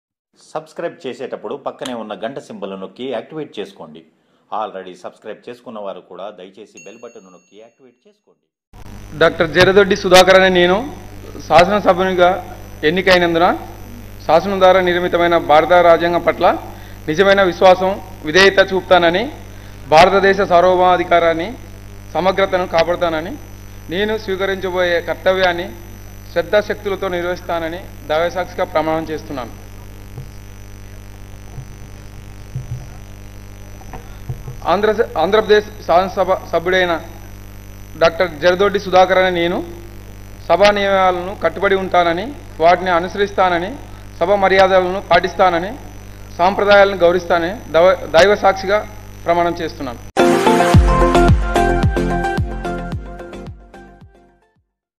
కోడుమూరు ఎమ్మెల్యేగా ప్రమాణ స్వీకారం చేసిన డాక్టర్ సుధాకర్ | Dr.Sudhakar Takes Oath as MLA in AP Ass
కోడుమూరు ఎమ్మెల్యేగా ప్రమాణ స్వీకారం చేసిన డాక్టర్ సుధాకర్ | Dr.Sudhakar Takes Oath as MLA in AP Assembly 2019 |APYUVA NEWS 12th June 2019..